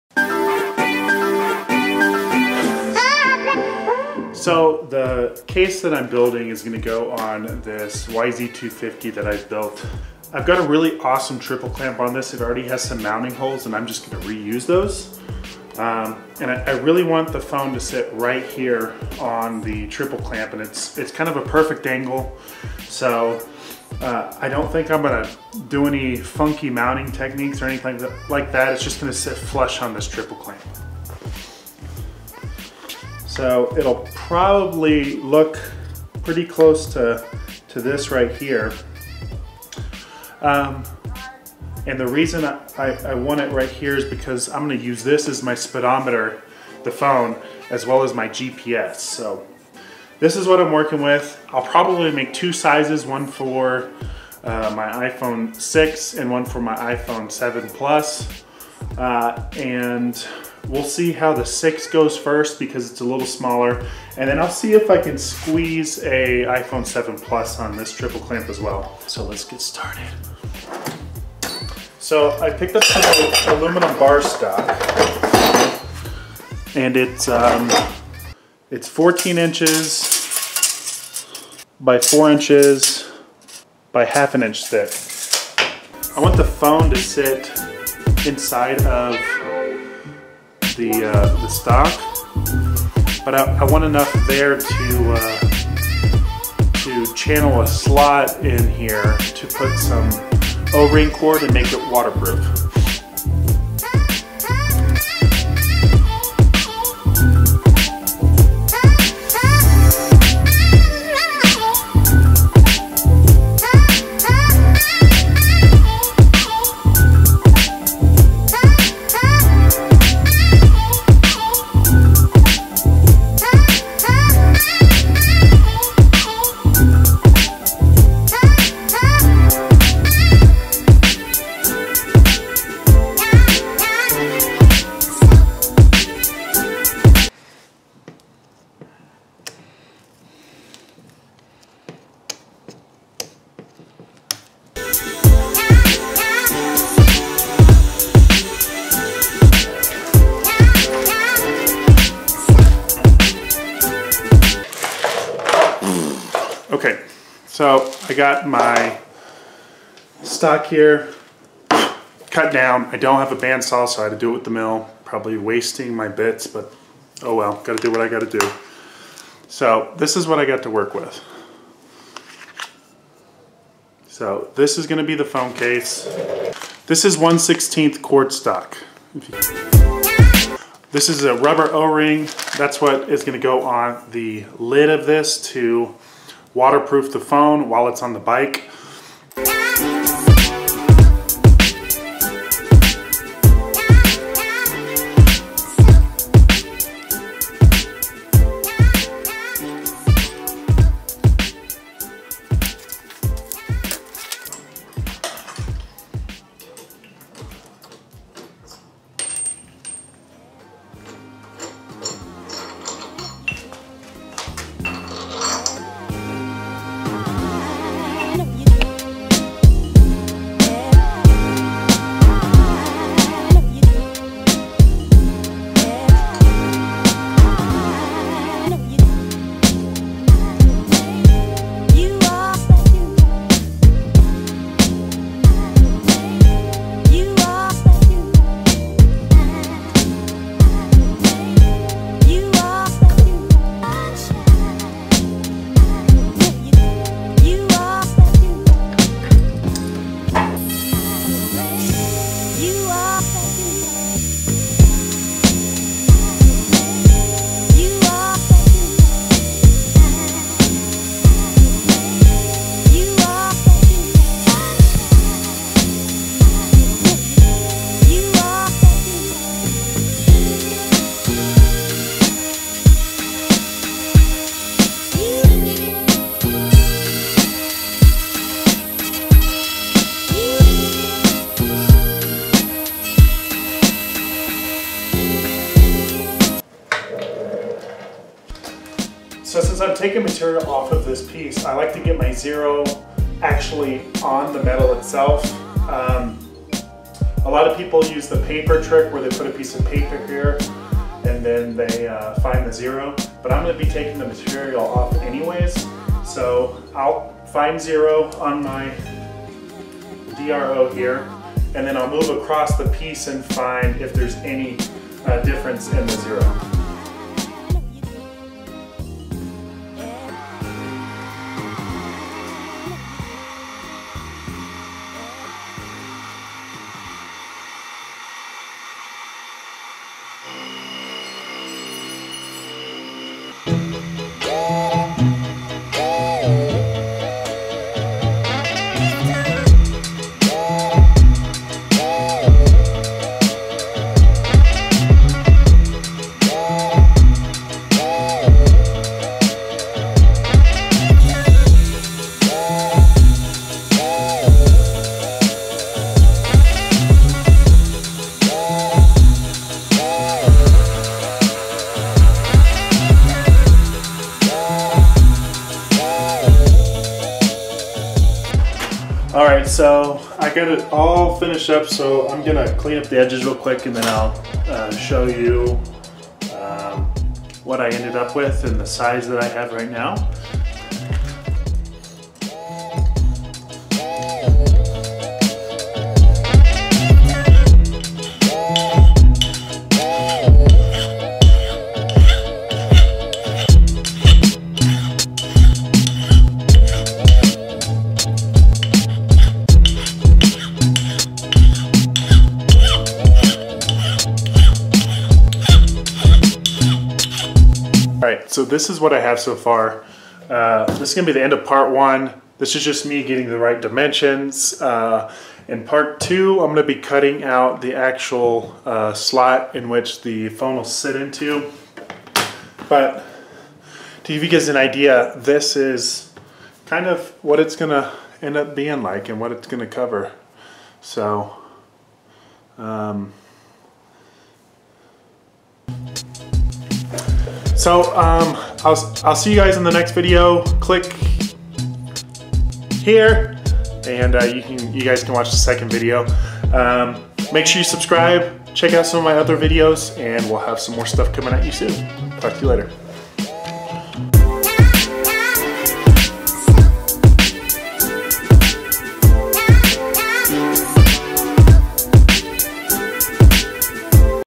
Aluminum Iphone Case Build Pt. 1
My first from scratch build with my brand new Grizzly g0758 mill. This is a mount for my iPhone 6 for my yz250. Made out of 6061 aluminum for a nice billet piece.

Check out and like my pages!